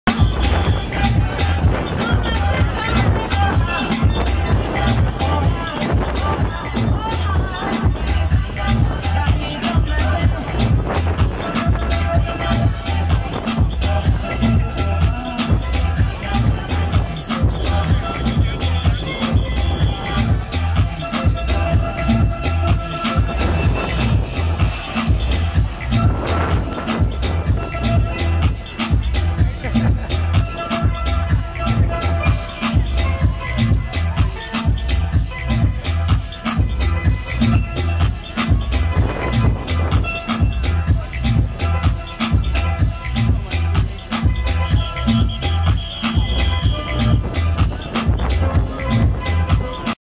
Best Dancing parrot EVER !!!!!!
Dancing at Coney island w10 on the boardwalk